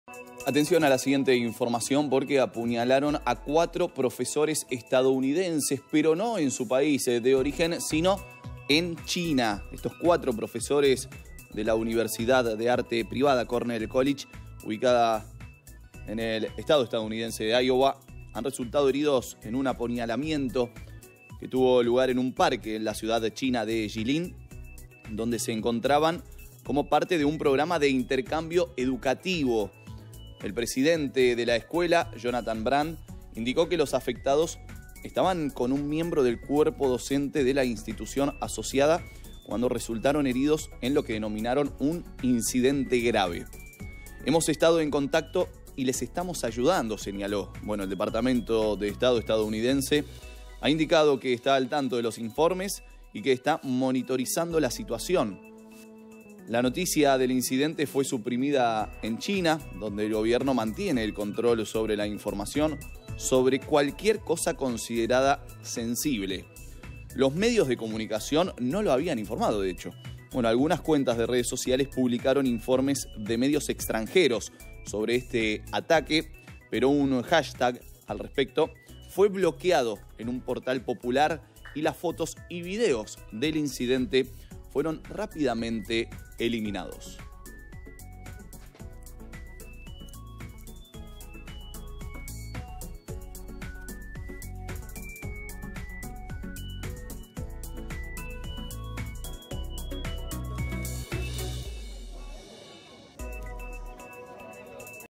CHINA | Los profesores fueron atacados en un parque de Jilin
👉 Cuatro profesores de la universidad de arte privada Cornell College, ubicada en el estado estadounidense de Iowa, han resultado heridos en un apuñalamiento que tuvo lugar en un parque en la ciudad china de Jilin, donde se encontraban como parte de un programa de intercambio educativo.